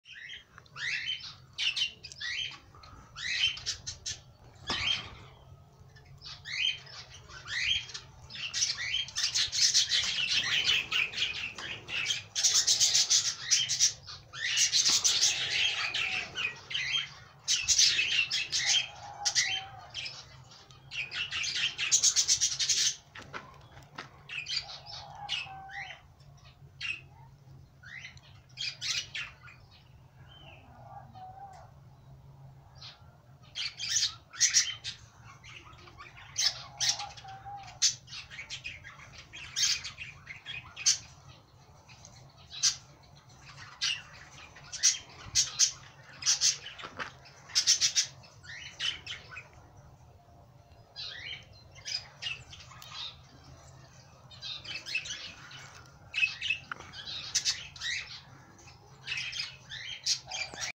Vögel im Klinikum Bremen Ost
Aufnahmen von der Voliere im Klinikum Bremen Ost mit ganz vielen Vögeln !